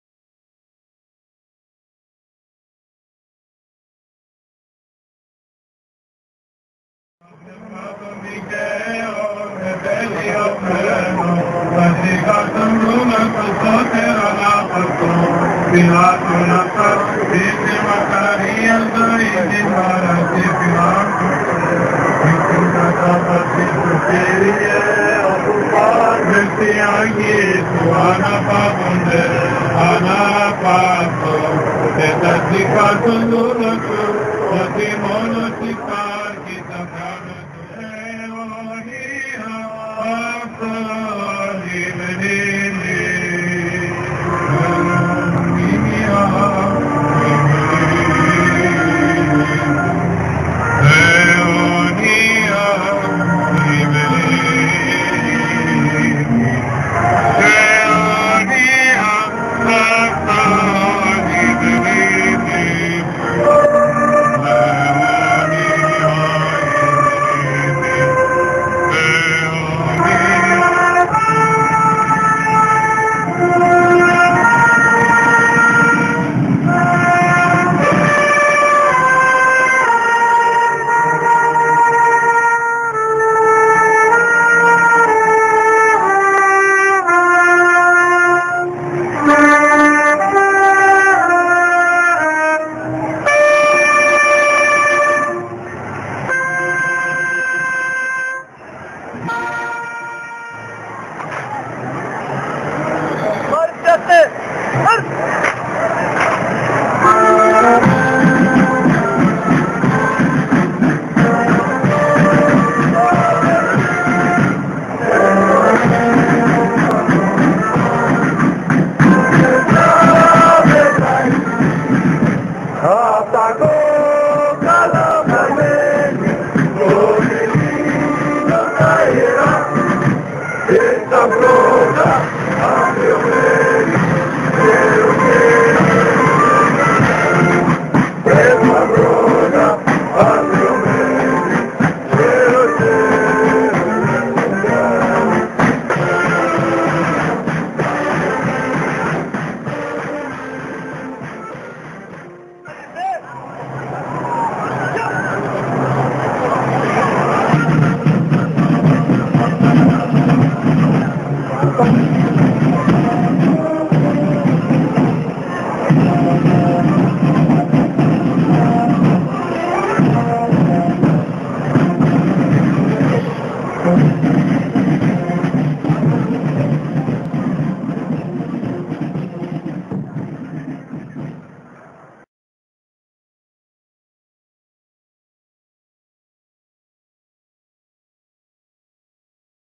Βέροια 28/10/2013 - Επιμνημόσυνη δέηση στο Ηρώο
~ Αποσπάσματα από την επιμνημόσυνη δέηση στο Ηρώο της πλατείας Ωρολογίου Βέροιας (Δευτέρα 28/10/2013)